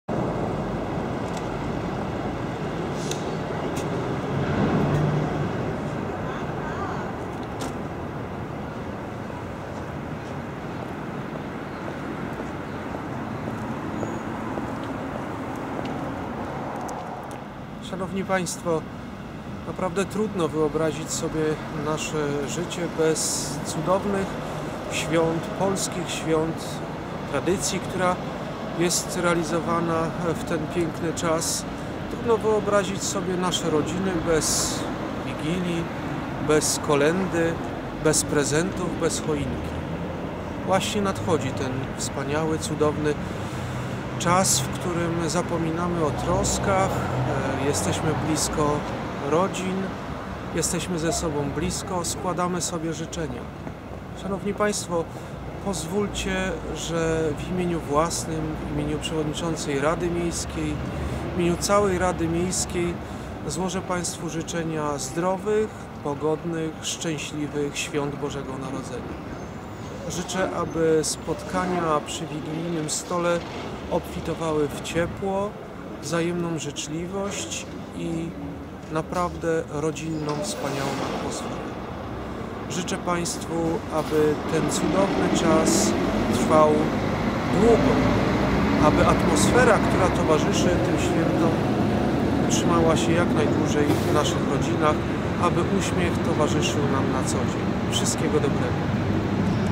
ŚWIĄTECZNE ŻYCZENIA Burmistrz Kcyni Piotra Hemmerlinga
Świąteczne życzenia Burmistrza Kcyni Piotra Hemmerliga. Boże Narodzenie 2013 r.